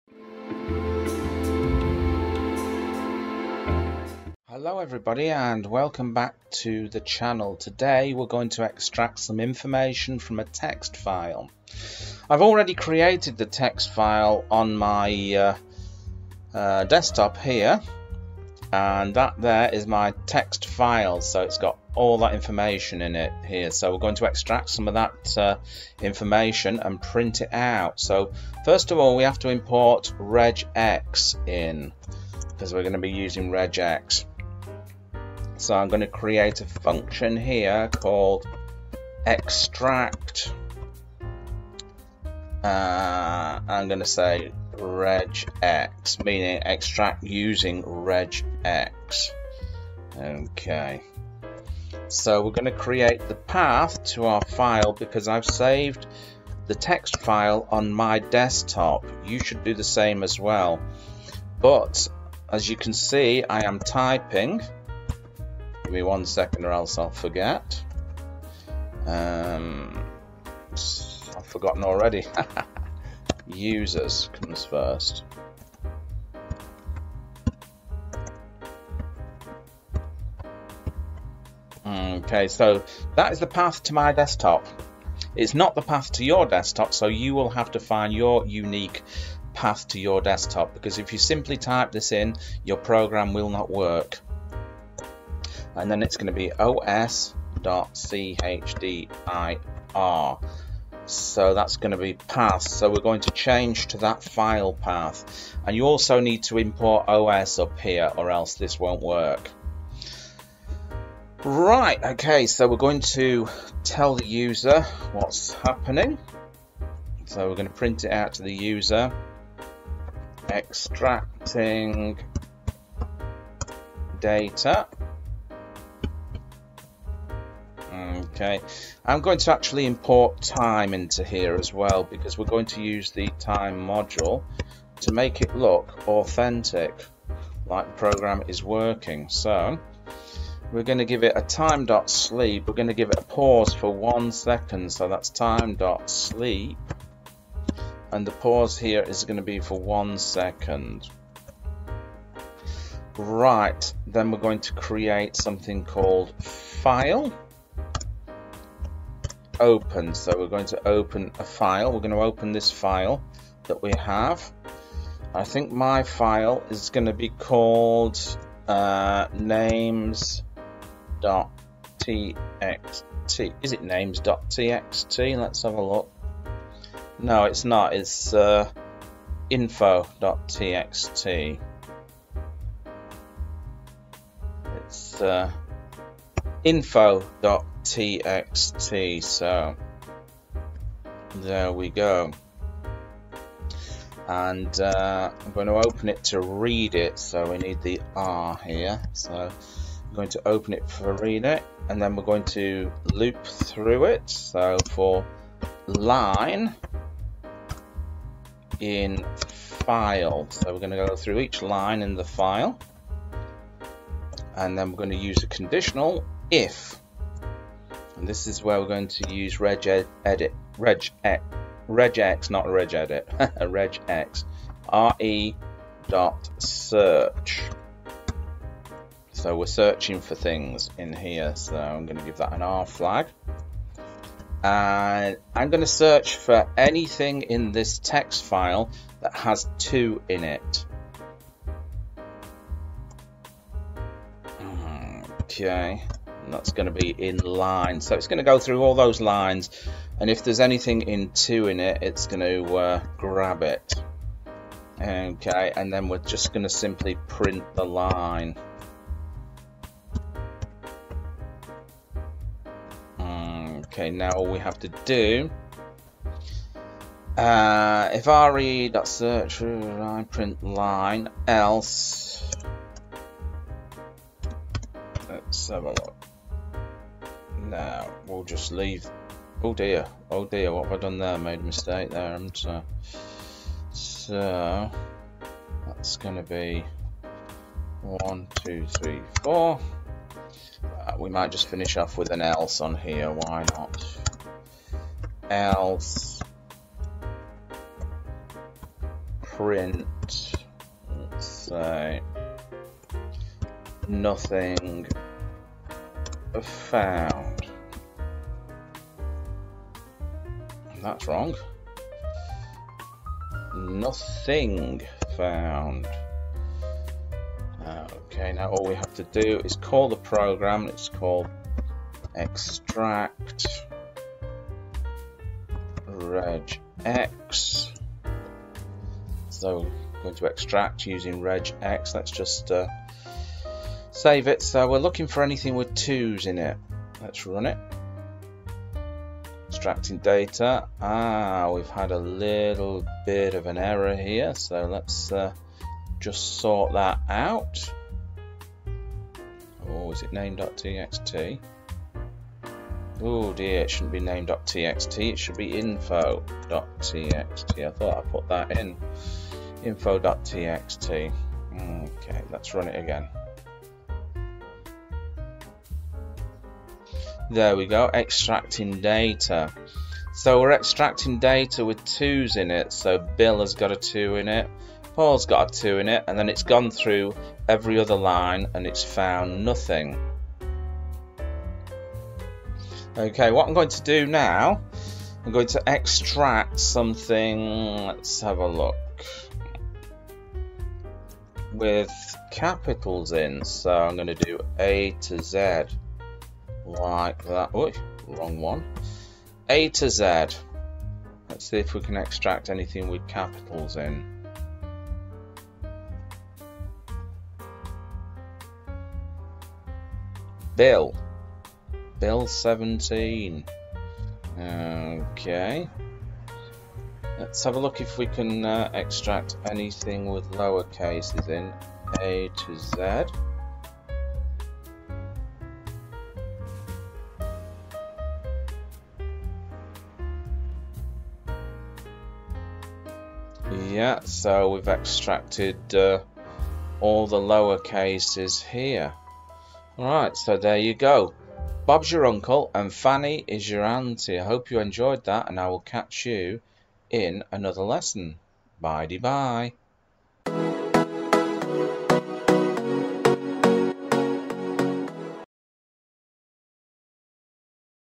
How To EXTRACT DATA From TXT File In Python 2023
Beginners Python Code Example Showing How To Extract Data From Text Files Using Python.